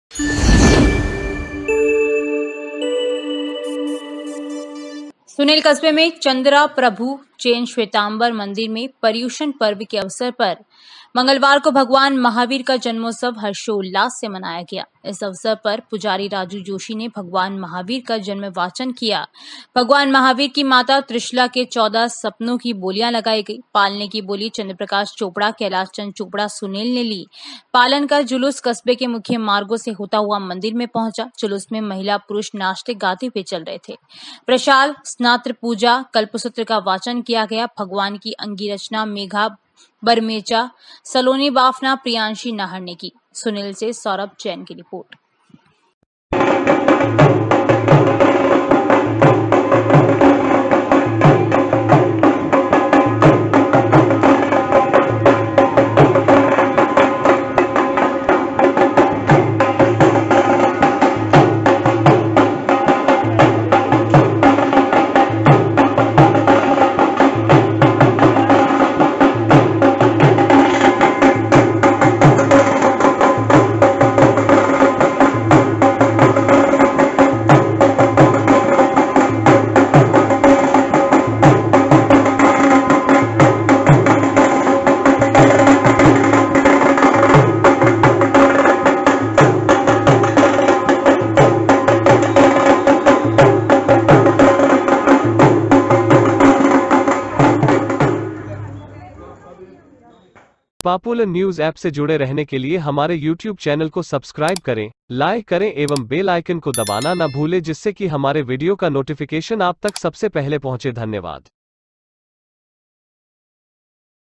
भगवान महावीर का जन्मदिन मनाया | Popular News App
सुनेल । चंद्राप्रभू जैन श्वेताम्बर मंदिर में पर्युषण पर्व के अवसर पर मंगलवार को भगवान महावीर का जन्मोत्सव हर्षोल्लास से मनाया गया। इस अवसर पर पुजारी राजू जोशी ने भगवान महावीर का जन्म वाचन किया। भगवान महावीर की माता त्रिशला के चौदह सपनों की बोलियंा लगाई गई। पालने की बोली चन्द्रप्रकाश चौपड़ा, कैलाशचंद चौपड़ा सुनेल ने ली। पालन का जुलुस कस्बे के मुख्य मार्गो से होता हुआ मंदिर में पंहुचा। जुलुस में महिला पुरूष नाचते गाते हुए चल रहे थे। प्रशाल, स्नात्र पूजा, कल्पसूत्र का वाचन किया गया। भगवान की अंगी रचना मेघा (माही) बरमेचा, सलोनी बाफना, प्रियांशी नाहर ने की।
सुनेल से सौरभ जैन की रिपोर्ट

rajasthantoday news